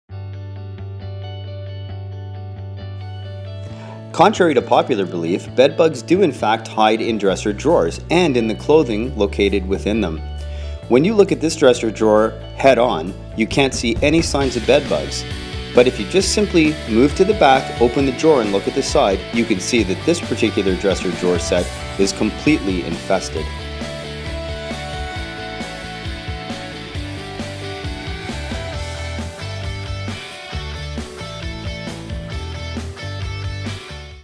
Bed bugs Toronto - Bed bugs hide in drawers too!
Bed bugs can easily hide where you can't find them! In this case, we found bed bugs hiding behind dresser drawers... No sign of bed bugs were visible when looking at this furniture head-on but a bit of persistance and knowing where to look revealed exactly where the bed bugs were hiding!

for more information.